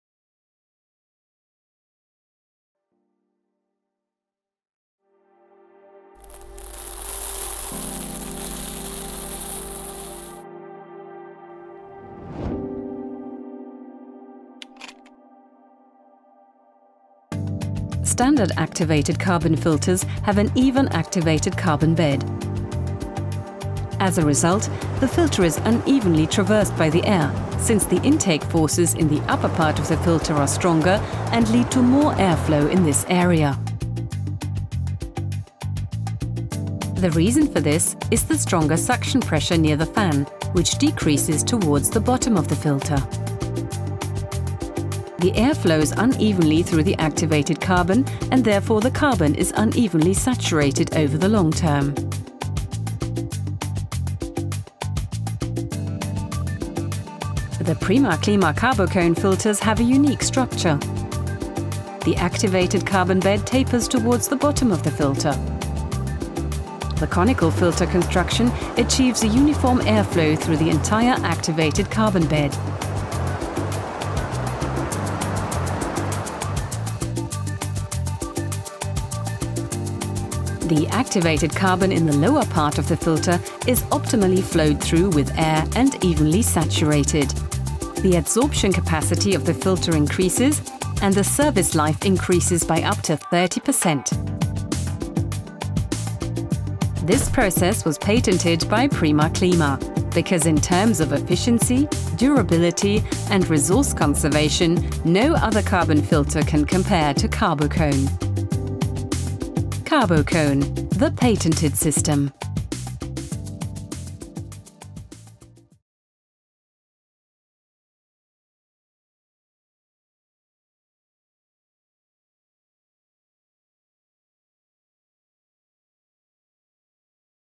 Carbocone - Activated Carbon Filters
The Prima Klima Carbocone filters have a unique structure. The activated carbon bed tapers towards the bottom of the filter. The conical filter construction achieves a uniform air flow through the entire activated carbon bed. The activated carbon in the lower part of the filter is optimally flowed through with air and evenly saturated. The adsorption capacity of the filter increases and the service life increases by up to 30%.

Standard activated carbon filters have an even activated carbon bed. As a result the filter is unevenly traversed by the air, since the intake forces in the upper part of the filter are stronger and lead to more air flow in this area. The reason for this is the stronger suction pressure near the fan, which decreases towards the bottom of the filter. The air flows unevenly through the activated carbon and therefore the carbon is unevenly saturated over the long term.

The patended system by Prima Klima. Nr.: EP2735354A1